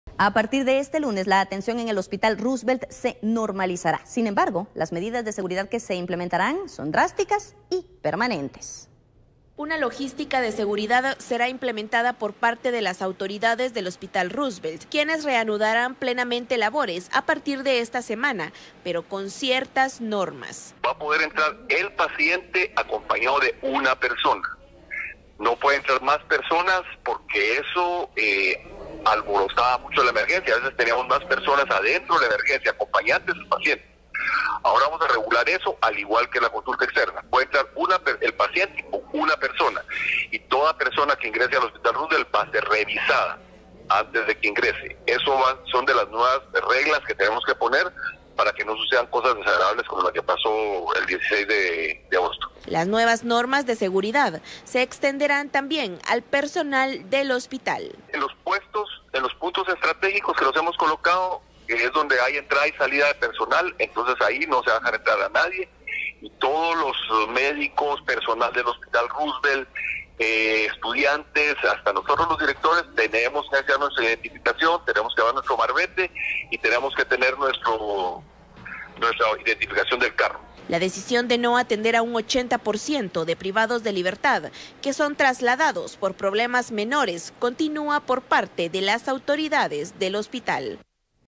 Refuerzan medidas de seguridad en el Hospital Roosevel
Atención será normalizada este lunes
La logística de seguridad es definida como drástica y permanente luego de la masacre ocurrida esta semana en el nosocomio.